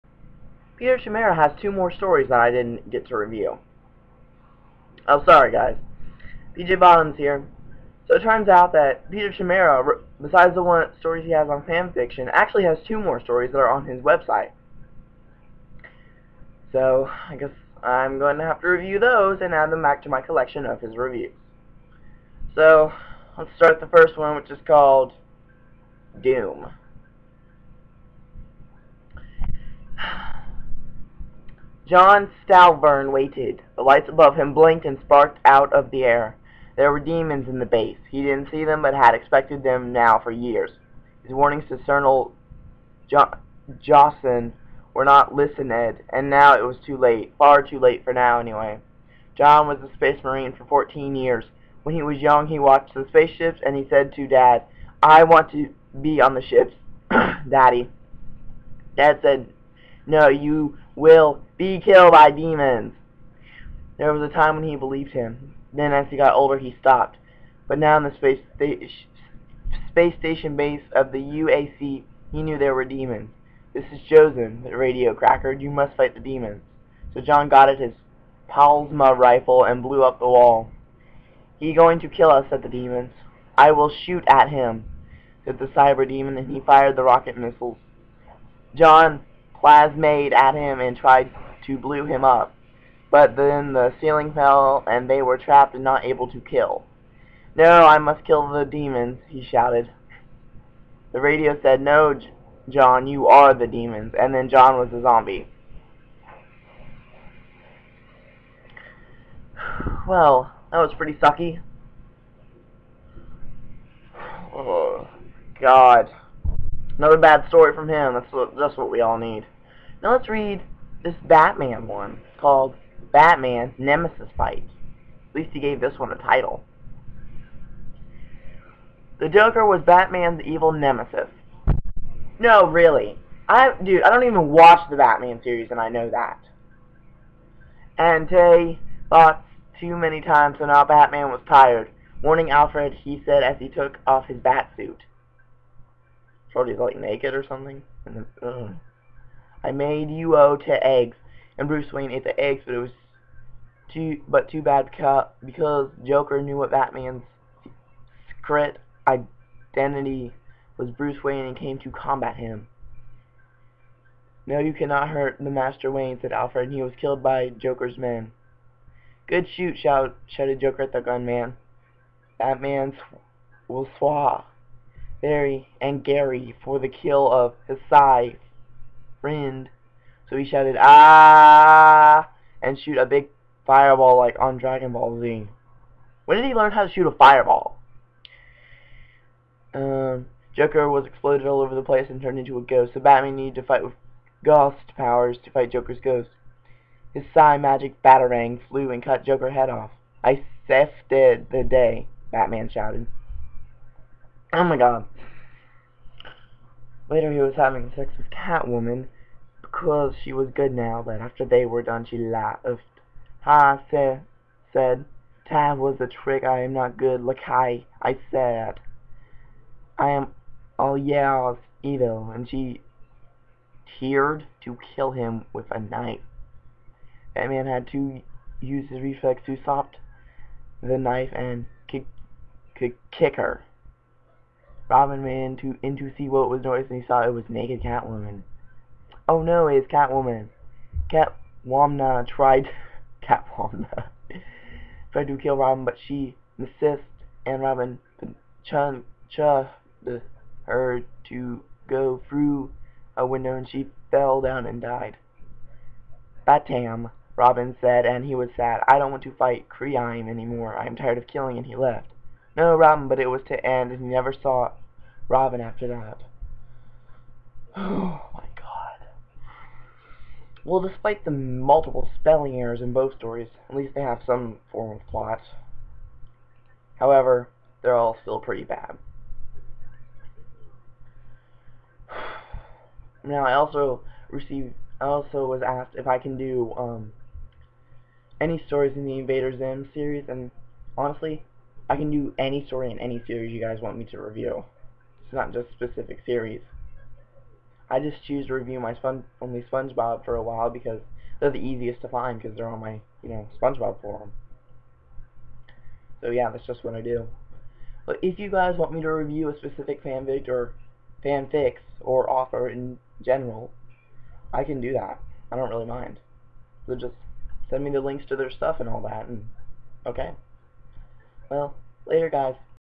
The PJ Bottomz Show, Episode 3 - Peter Chimaera (13th & 14th)
PJ Bottomz reviews a horrible author on fanfiction, Peter Chimaera.